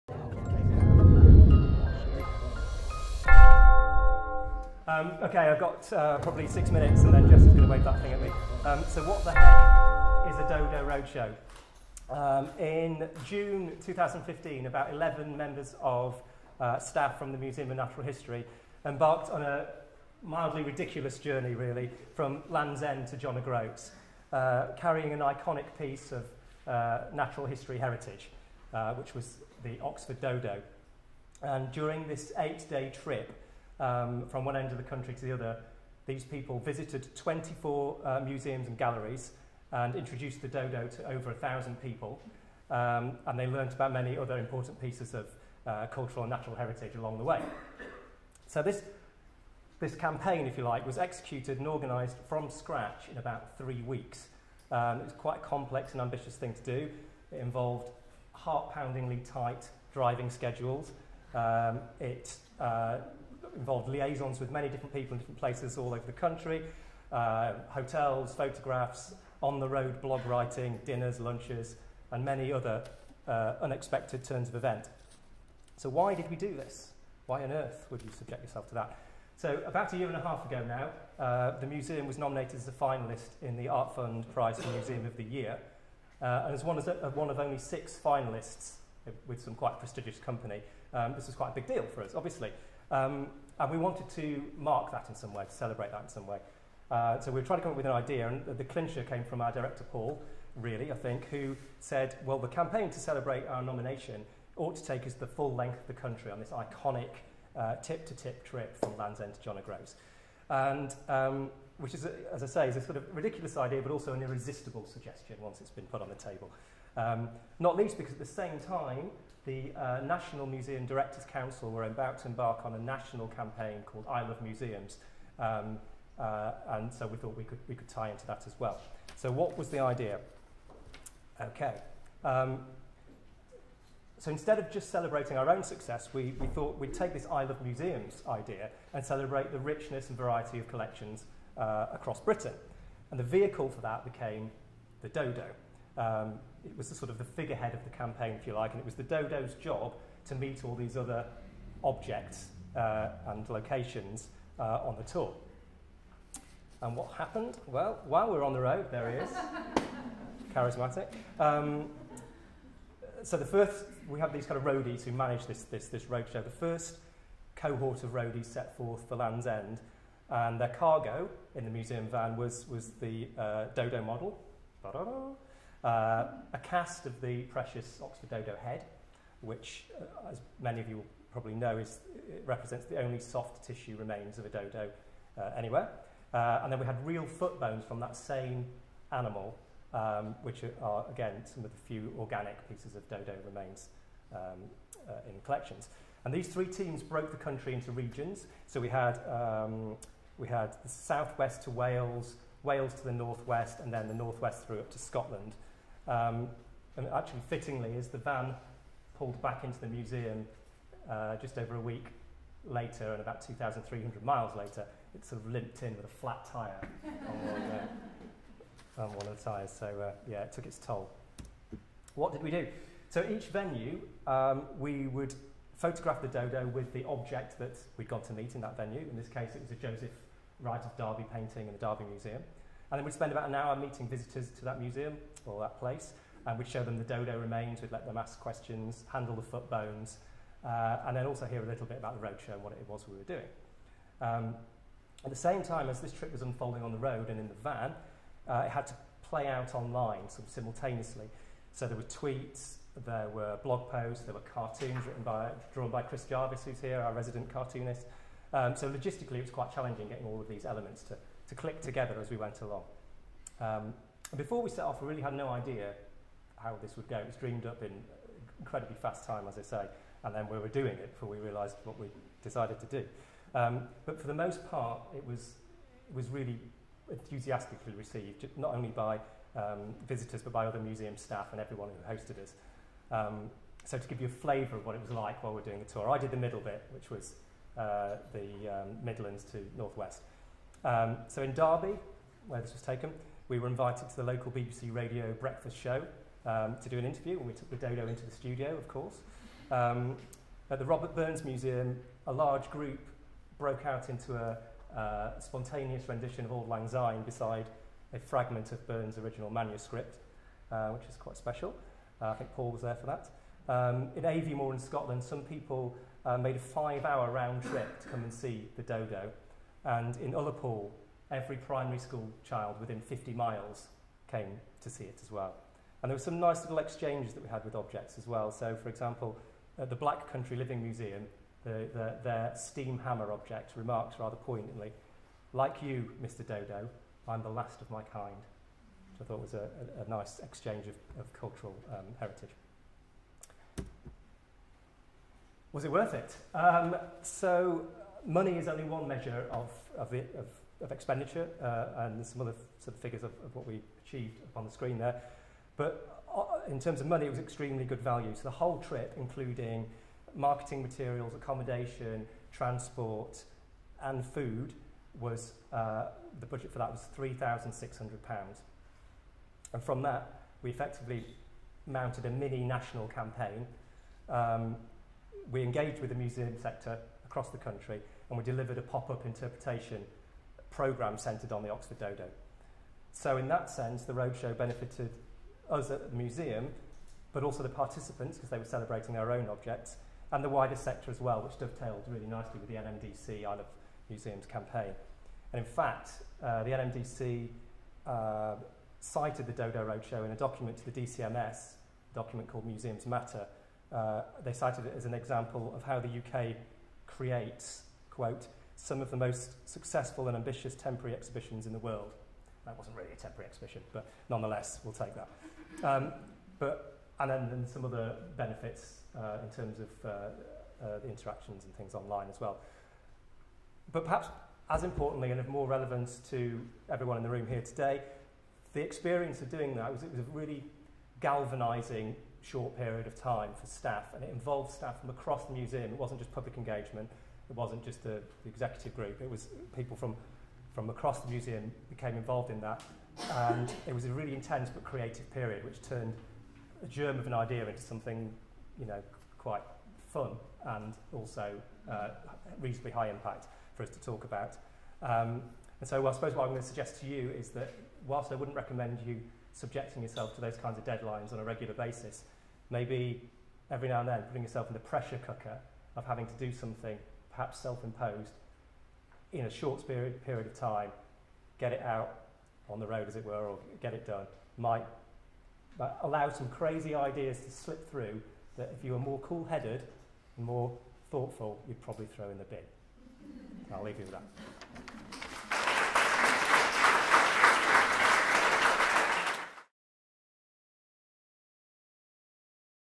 Dodo Roadshow
Scott Billings, Digital Engagement Officer, Museum of Natural History at the Oxford University Museums Staff Conference, September 2016.

The Dodo Roadshow was a crazy tip-to-tip trip across Britain with one of natural history's most iconic specimens. Scott talks about how the idea was developed from an initial suggestion around the director's table to their arrival at John O'Groats barely one month later, having visited collections in 22 museums and galleries along the way.